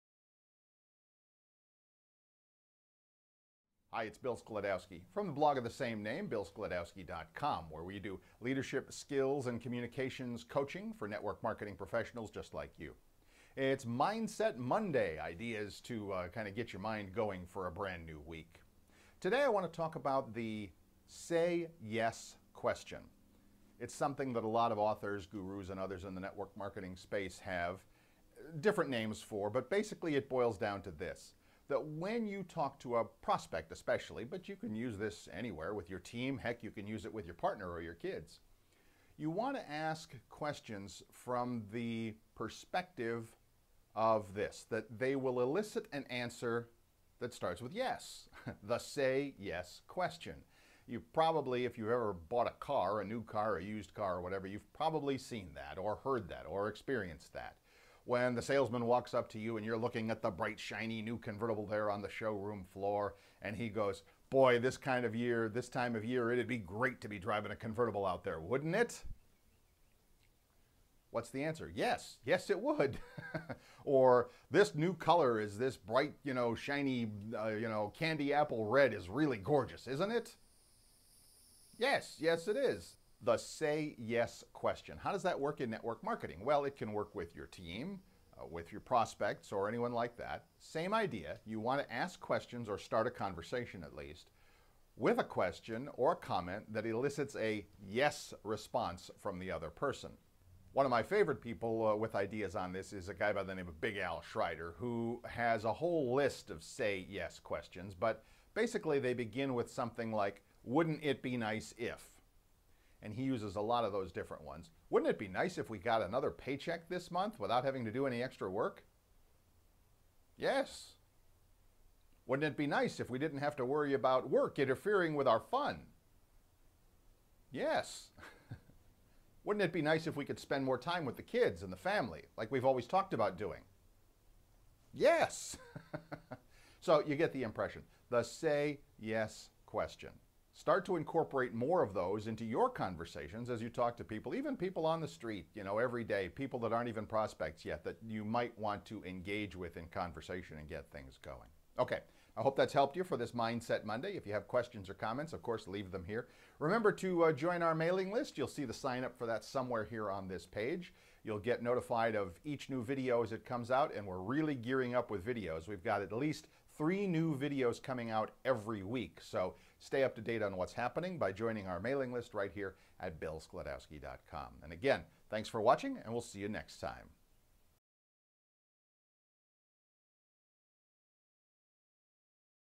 Finding Your WHY In Life and Business
- Leadership Skills Development and Communication Coaching for Network Marketing Professionals

This is what we call a "Mindset (or) Motivation Monday video. This one is all about cultivating a way of using "Say Yes" questions. Using them with prospects, team members and others (even your spouse, partner or children.)

Network marketing
MLM
Young Living
Home-based business
Prospecting
Mindset and Motivation
Work from home
Leadership
Essential oils